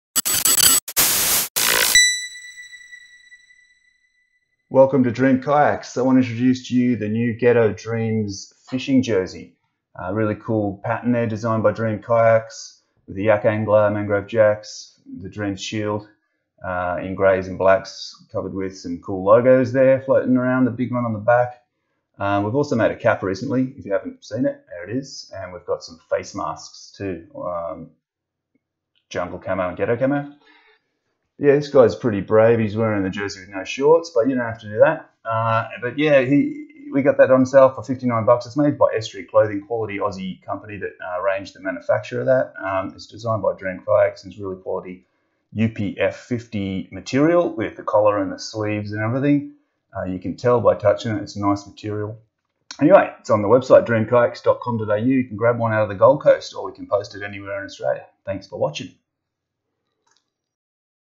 Ghetto Dreams Fishing Jersey
Introducing our new "Ghetto Dreams" fishing jersey available on our website for just $59. It's UPF 50 and designed 100% by Dream Kayaks with the Yak angler Camo in the Ghetto Camo colours! Manufactured care of Estuary Clothing. Top quality garment with style to boot!